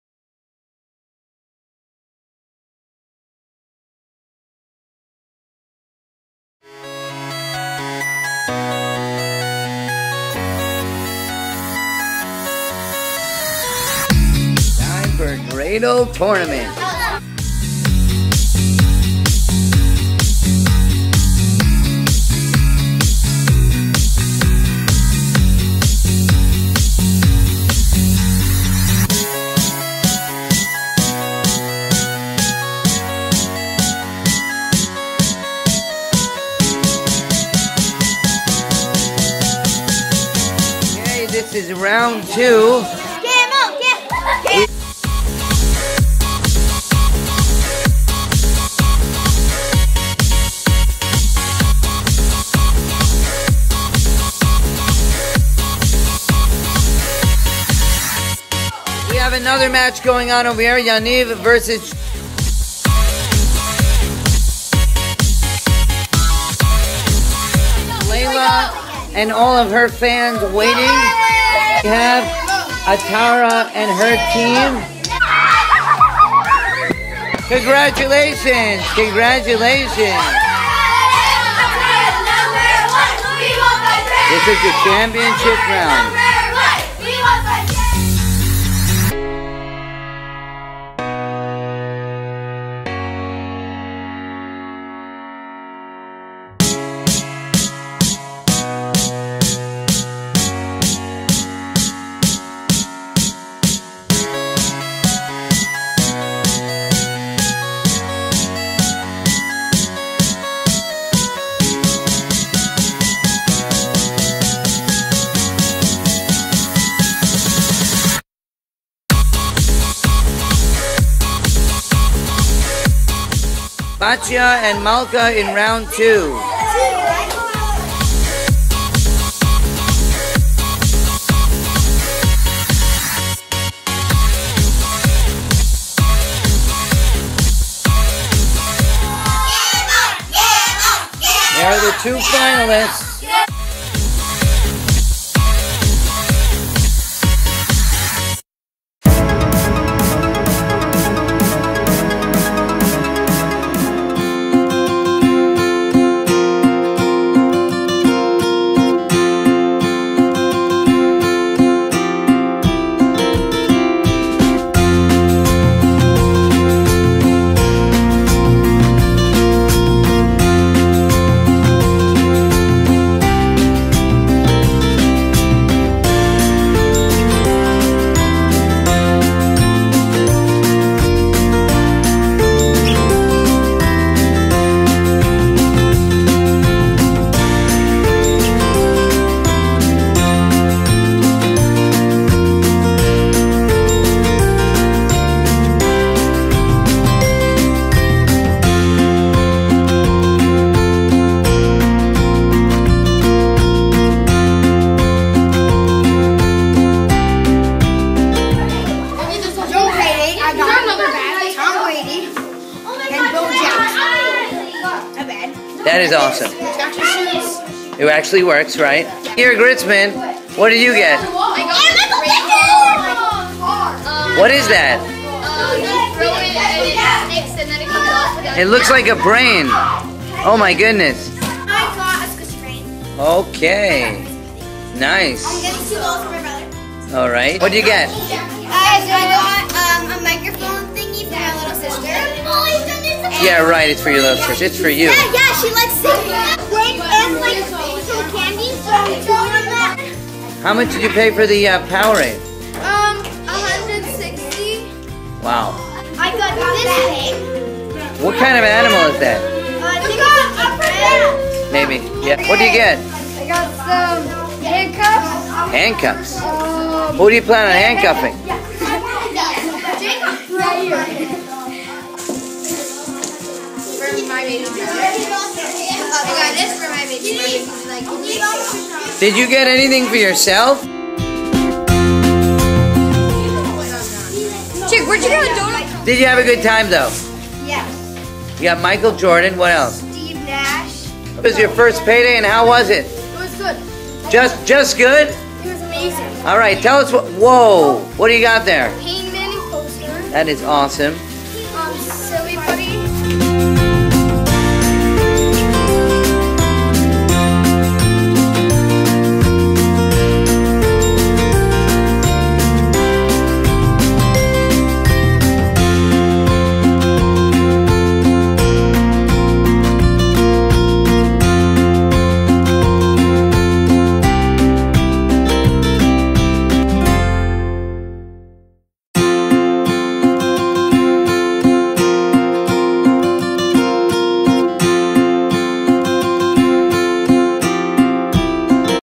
"Memories"
Take a peak at some of the highlights from Friday's Payday event and Chanuka's Dreidel Tournament.